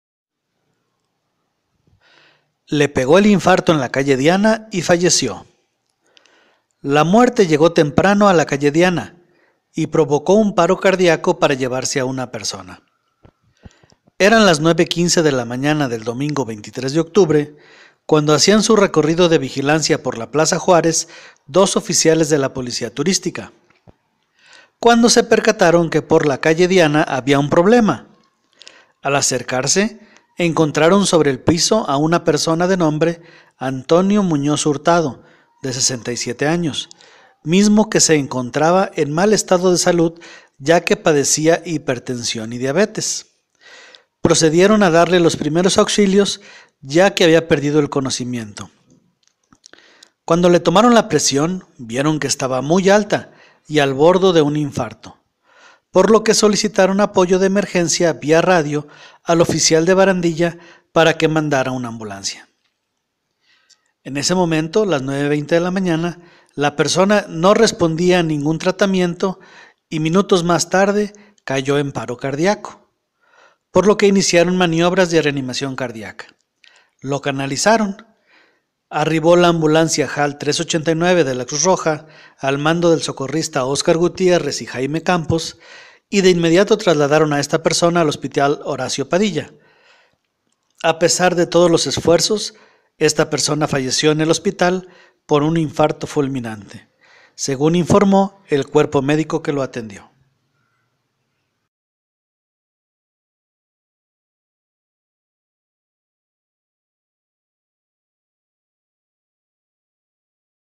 Le Pegó el Infarto en la Calle Diana y Falleció
La muerte llegó temprano a la calle Diana y provocó un paro cardiaco para llevarse a una persona. Eran las 9:15 a.m. del domingo 23 de octubre cuando hacían su recorrido de vigilancia por la Plaza Juárez dos oficiales de la Policía Turística, cuando se percataron que por la calle Diana había un problema, al acercarse encontraron sobre el piso a una persona de nombre Antonio Muñoz Hurtado, de 67 años, mismo que se encontraba en mal estado de salud ya que padecía hipertensión y diabetes, procedieron a darle los primeros auxilios ya que había perdido el conocimiento, cuando le tomaron la presión vieron que estaba muy alta y al bordo de un infarto, por lo que solicitaron apoyo de emergencia vía radio al oficial de barandilla para que mandaran una ambulancia, pues para ese memento, las 9:20 a.m., la persona no respondía a ningún tratamiento y minutos más tarde cayó en paro cardíaco, por lo que iniciaron maniobras de reanimación cardiaca y lo canalizaron; arribó la ambulancia Jal-389 de la Cruz Roja al mando del socorrista Oscar Gutiérrez y Jaime Campos que de inmediato trasladaron a esta persona al hospital Horacio Padilla. A pesar de todos los esfuerzos esta persona falleció en el hospital de infarto fulminante, según informó el cuerpo médico que lo atendió.